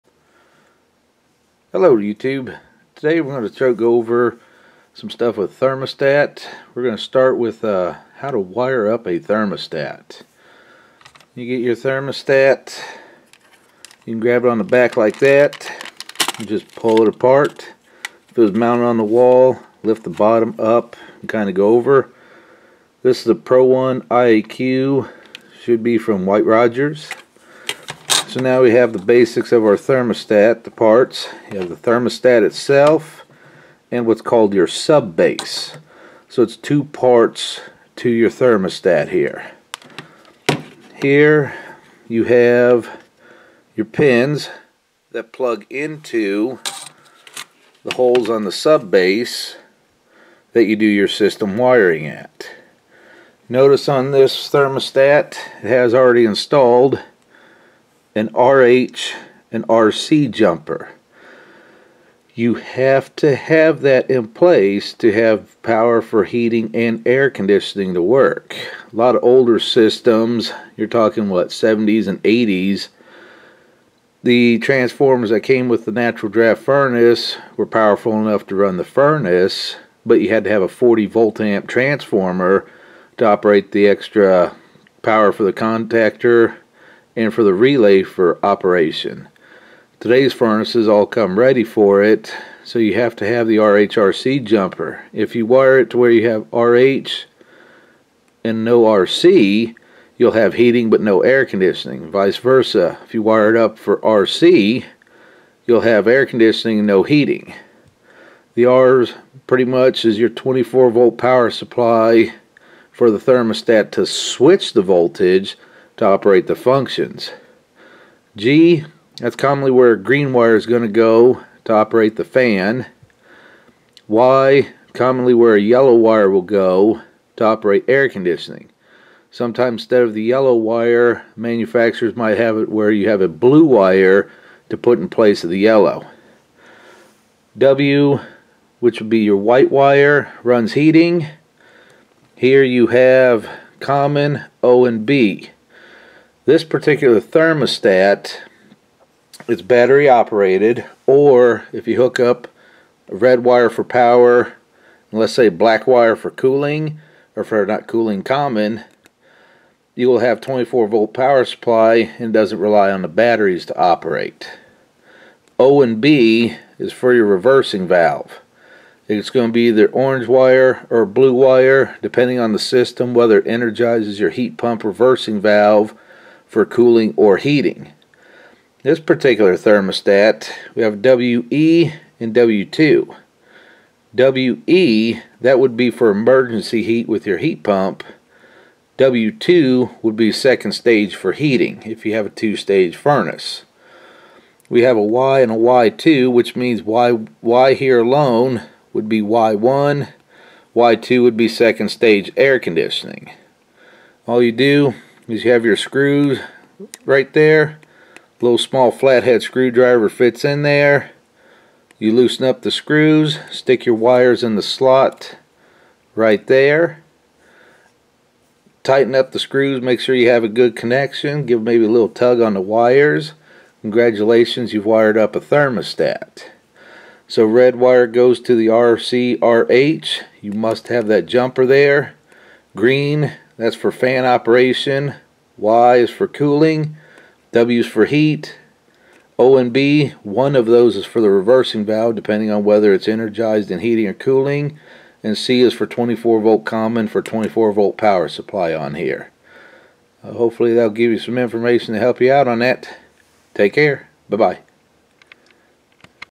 Wiring a thermostat for any furnace any thermostat
This video gives the color code, what functons do what, and just a general discussion on wiring a thermostat. I did forget to say in the video to ensure that the furnace R G Y W wires match the thermostat.

There is no differnce between the wiring of a bi-metal thermostat or a fancy digital or programable stat. The R G Y W will be the same and the functions are the same regaurless of thermostat brand or equipment.

Before you begin workig on any wire or anything electrical, ensure that the power supply is turned off. Please consult a profesional if you do not understand wireing or how things work.